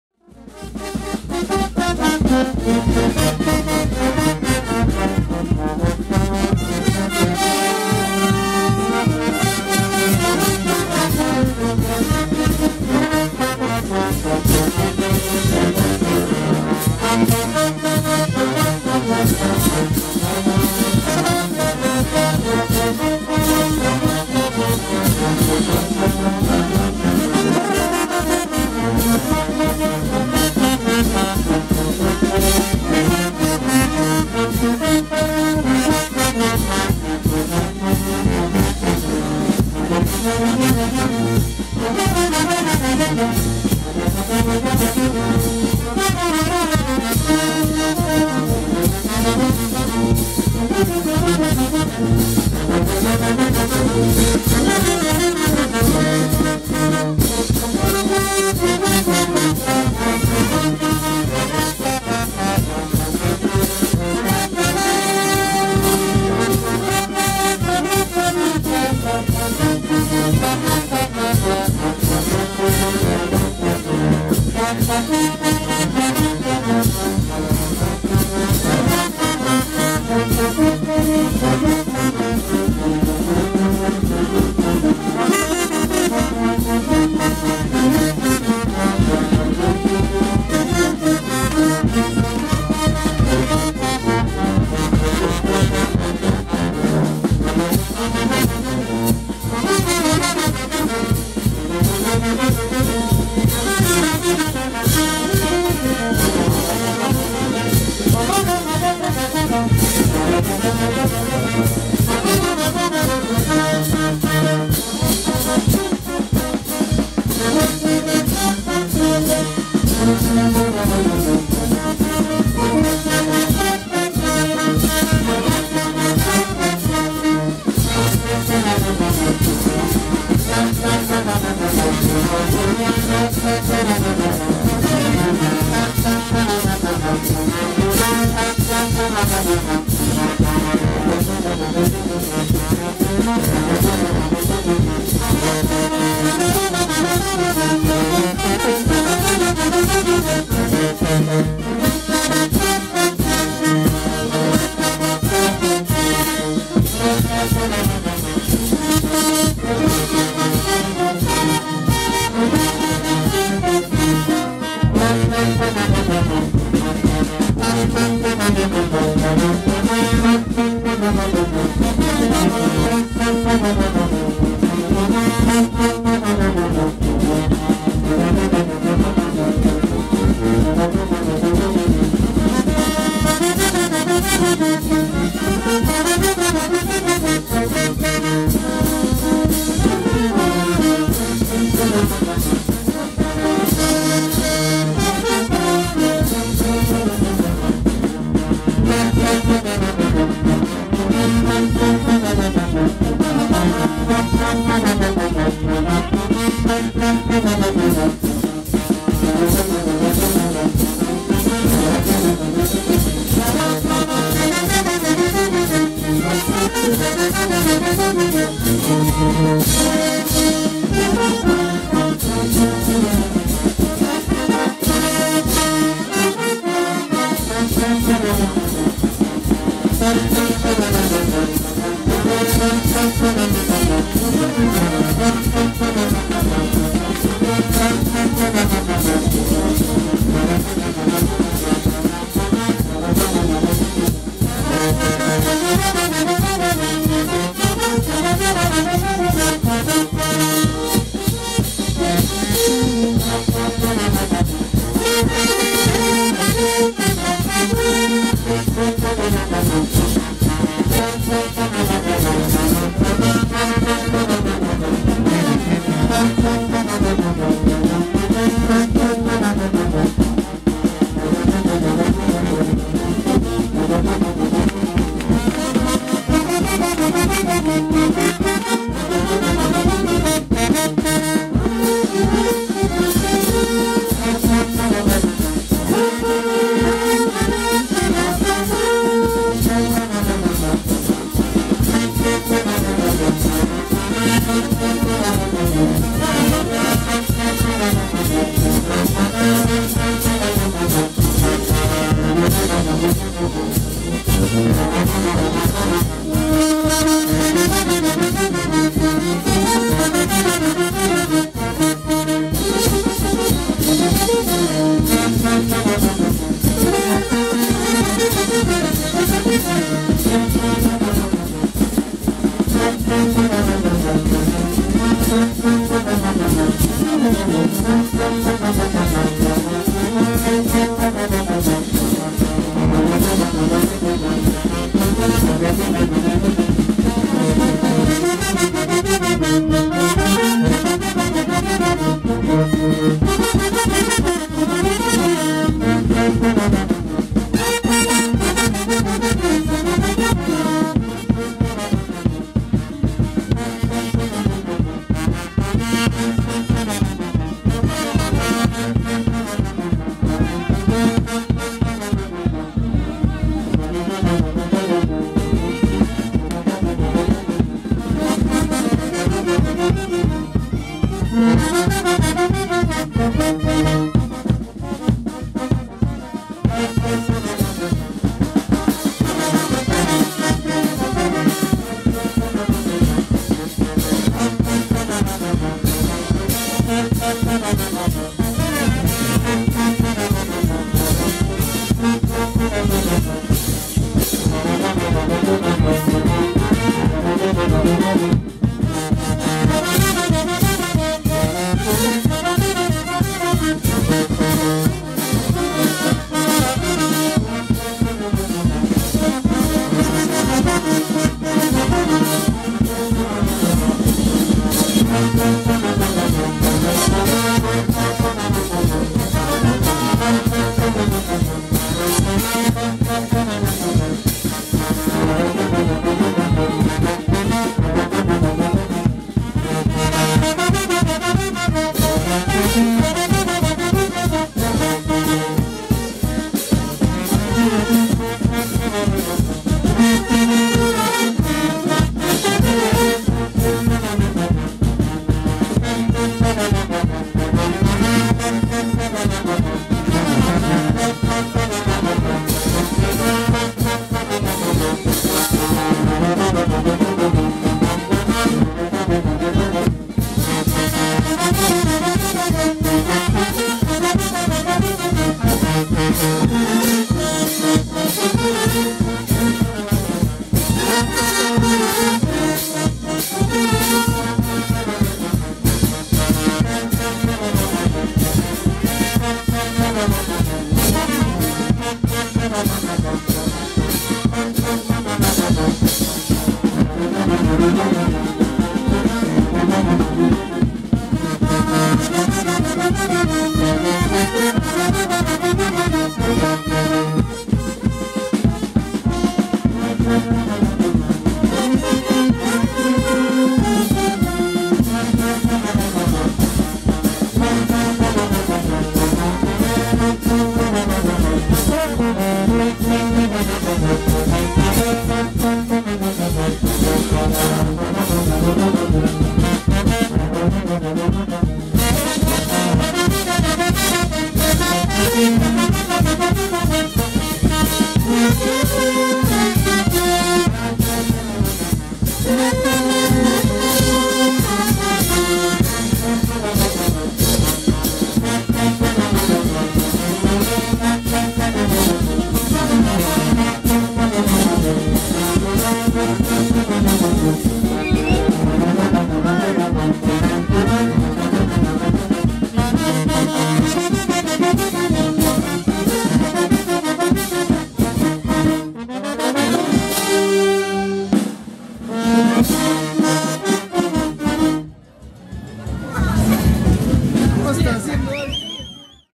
ISSA SUPER SONORA ACOLLINA, OFRESO YAULINO,
OFRESO YAULINO AL TAYTA AUQUISH, BENDICIONES POR LA TRADICION A SU NOMBRE. MUSICA A CARGO DE INTERNACIONAL SUPER SONORA ACOLLINA. ISSA